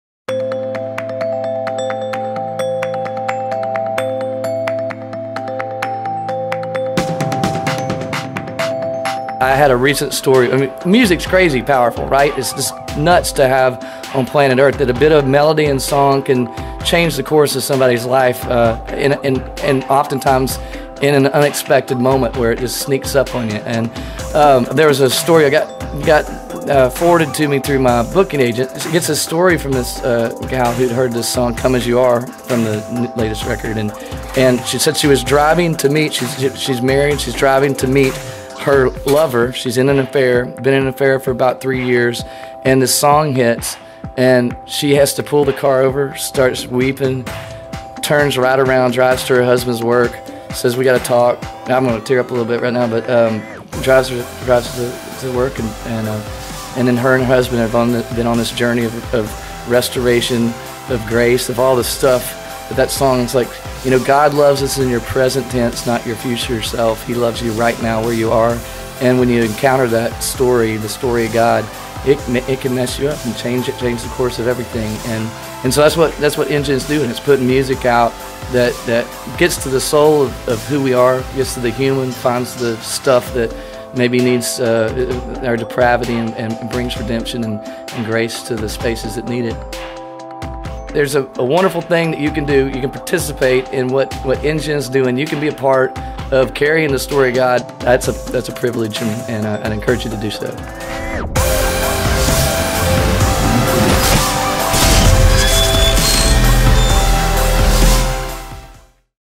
Crowder Shares how YOU Power a World of Good
When you support NGEN, you're spreading hope and changing lives! Watch as Crowder shares how you're making a difference.